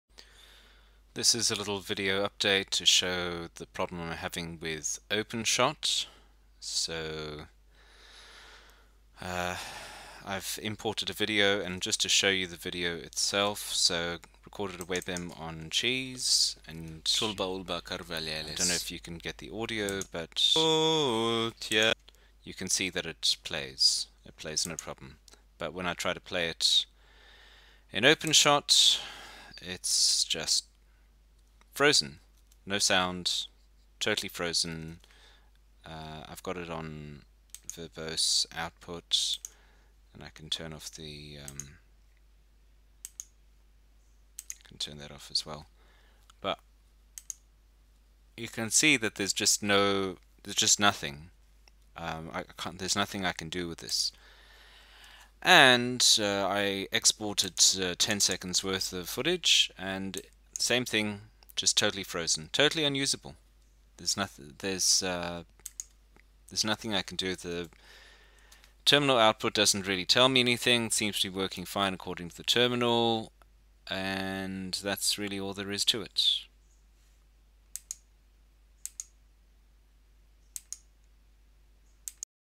Openshot 2.4.1 still unusable
Openshot 2.4.1
Ubuntu 16.04
Installed from PPA
32GB RAM